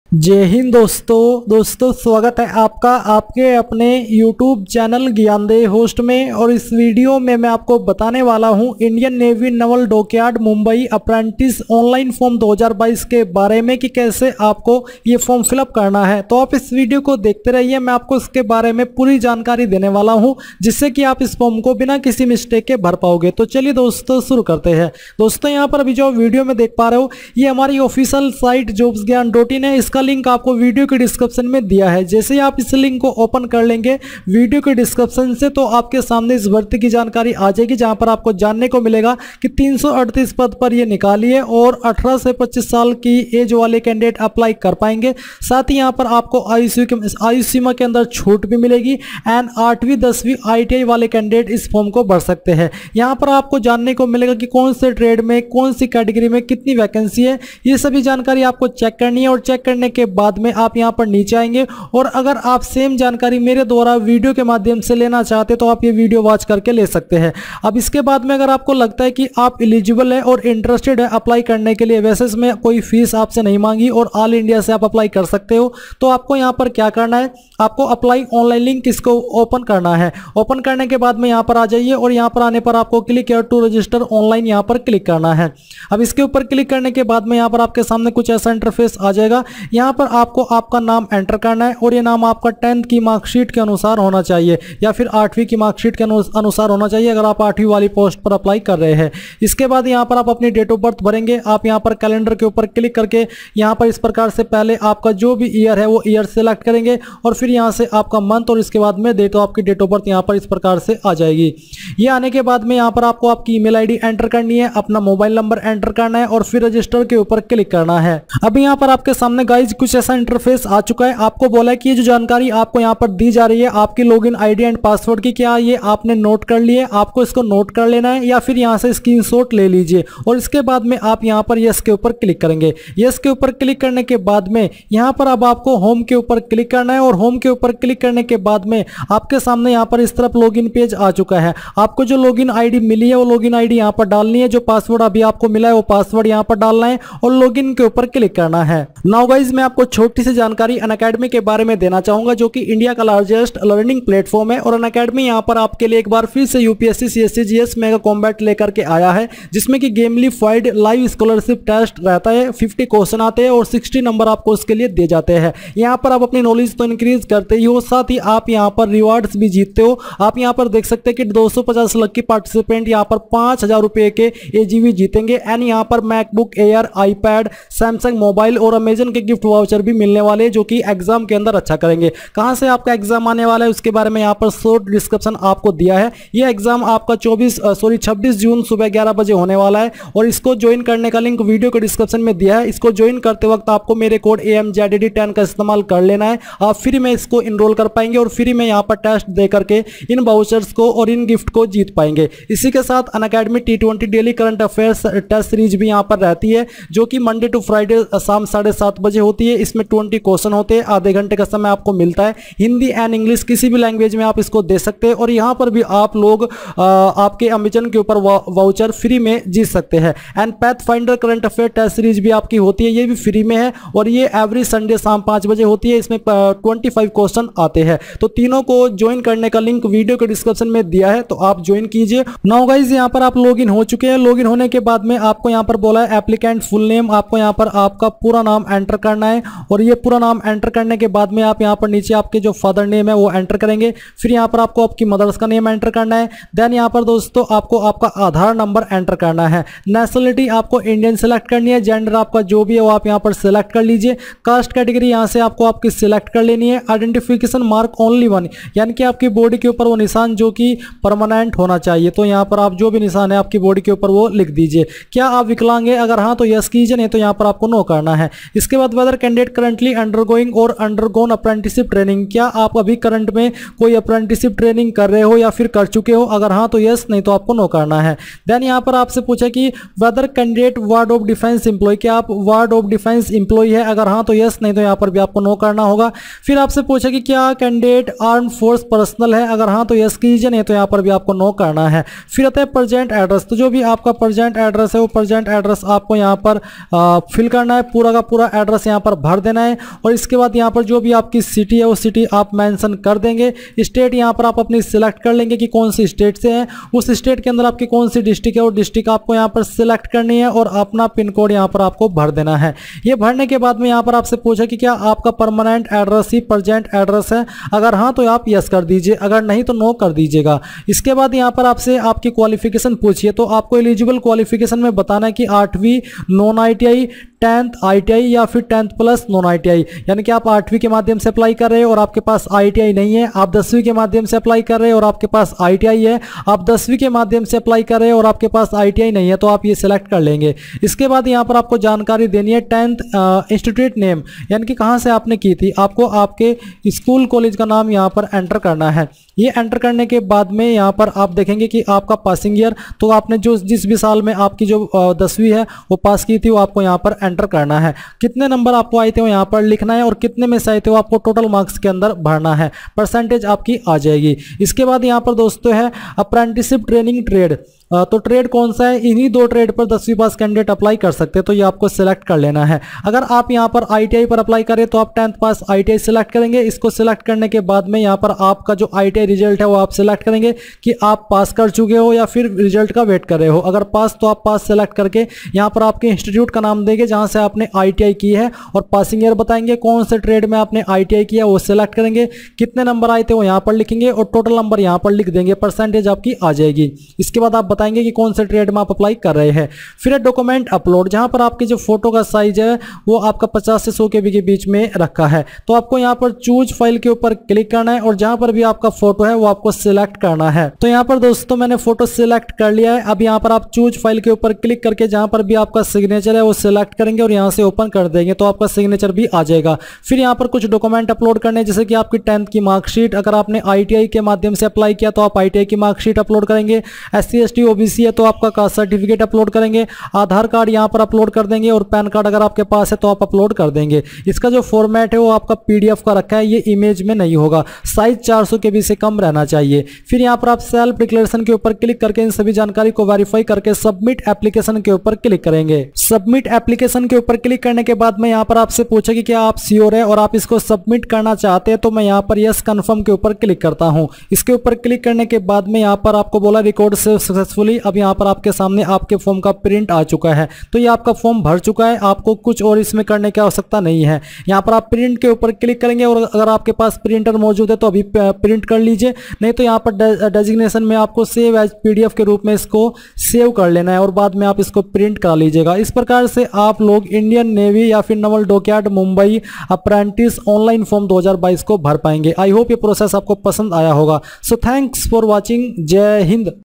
Indian Navy Apprentice Online Form 2022 | Naval Dockyard Trade Apprentice Online Form 2022
The Indian Navy Apprentice Online Form 2022 Apply Link has been announced by the Indian Navy, Naval Dockyard. Candidates also search it as Indian Navy Tradesman Bharti 2022. Apply online for Indian Navy Tradesman Vacancy 2022 Applicants who want to apply for Indian Navy Group C Notification 2022 should check out the complete details.

Aapka Dost
GyanDev Host